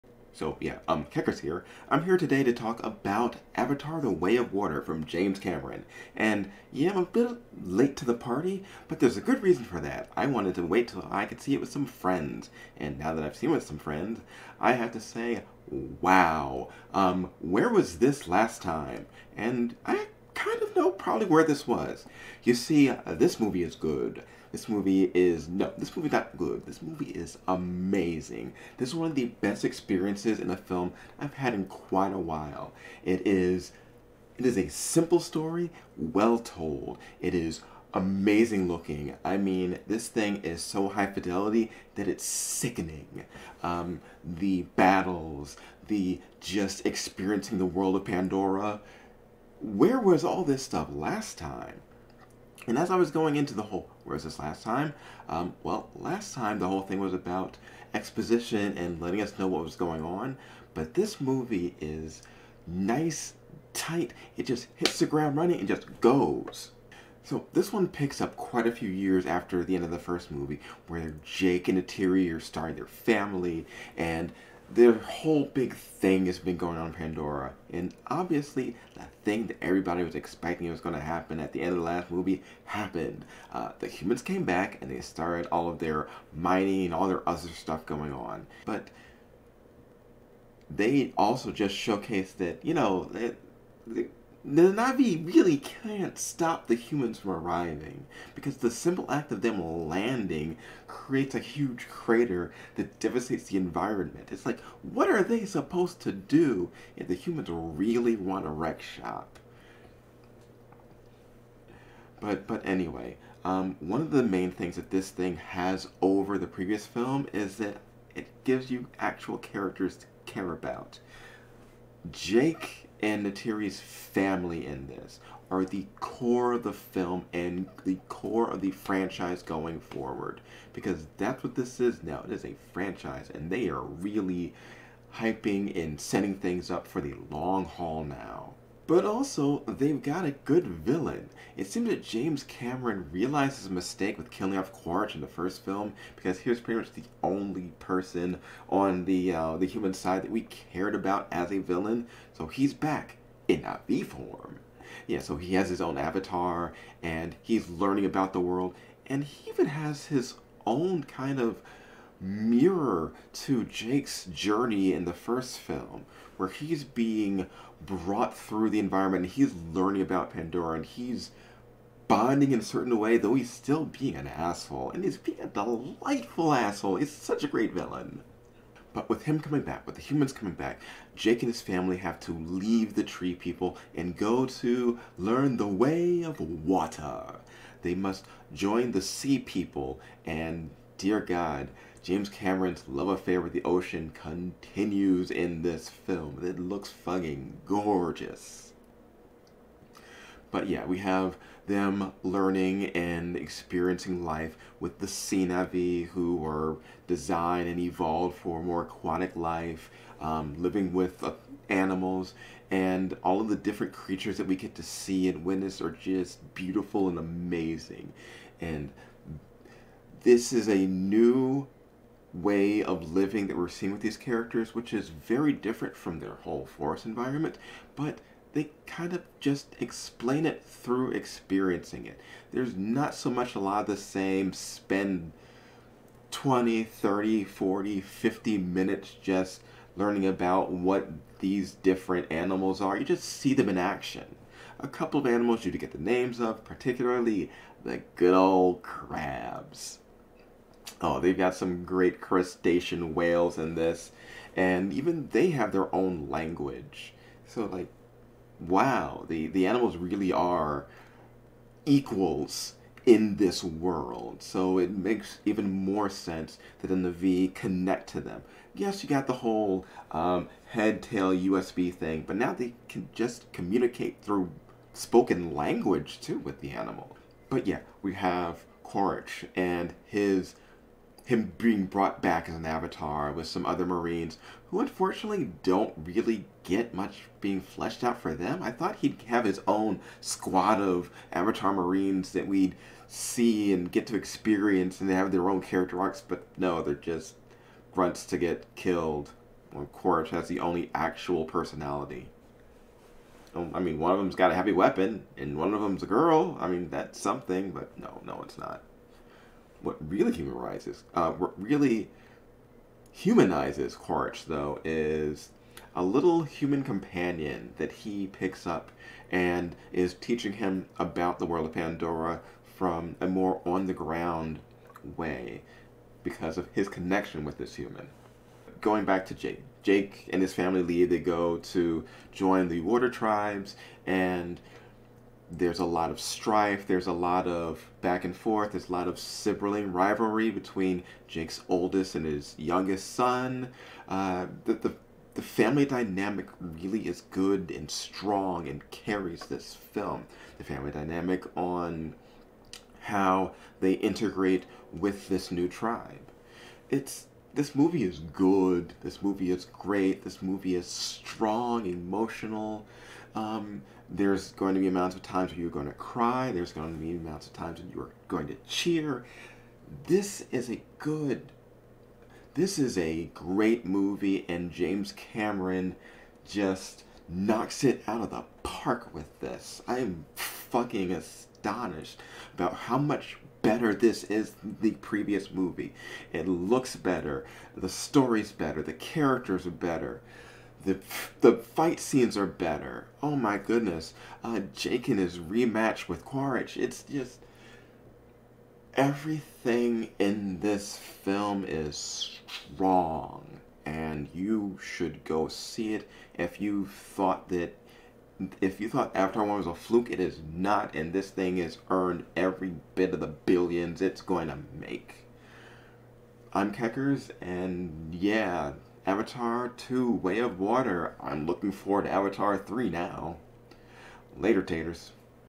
Belated Avatar 2 Review
If you haven't watched Avatar 2 by now I'm probably not going to convince you, but if you haven't and are just a fan of mine. First of all Thanks But yeah the movie's good